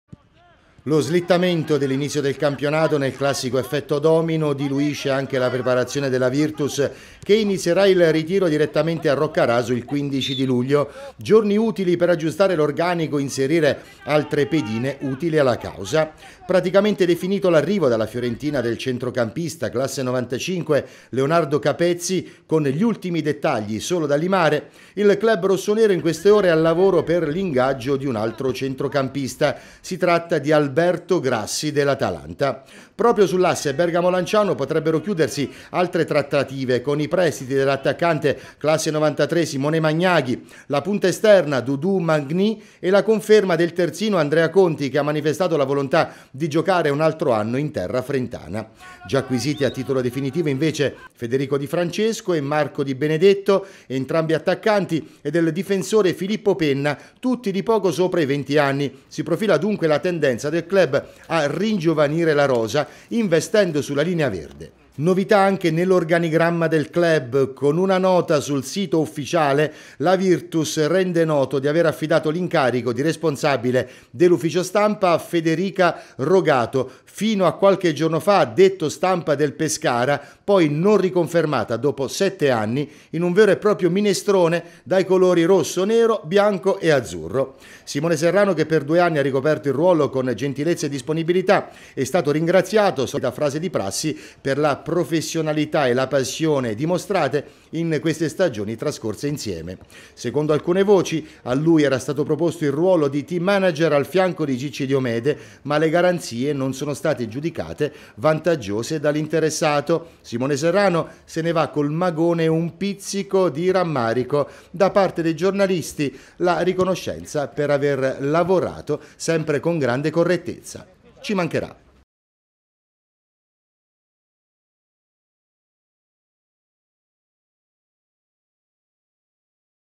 Serie B: la Virtus cambia pelle
Lanciano, servizio Tgmax 9 luglio 2015. Serie B: si impenna il calciomercato della Virtus Lanciano con tre acquisti dall’Atalanta, a breve in rossonero Andrea Conti, Simone Magnaghi e Doudou Mangni. L’ufficio stampa è affidato a Federica Rogato, per 7 anni al Pescara.